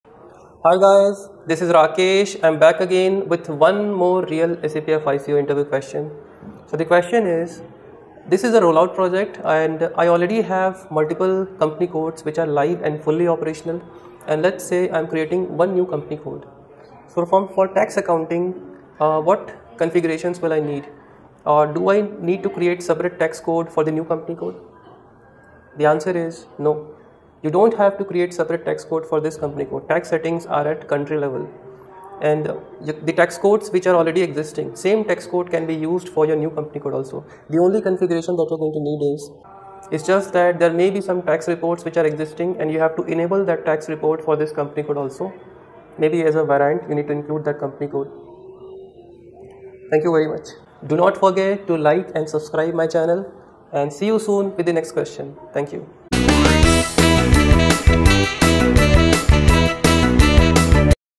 SAP FICO Interview Question & Answer | In a roll out project what configuration is needed for tax?
Welcome to SAP FICO interview preparation series !

Question is: In a roll out project, you are creating a new company code. What configuration you need to do for tax accounting.

Answer is: Since live company codes already exist in the country hence no new tax configuration is needed for new company code. Existing tax codes will be used in new company code also.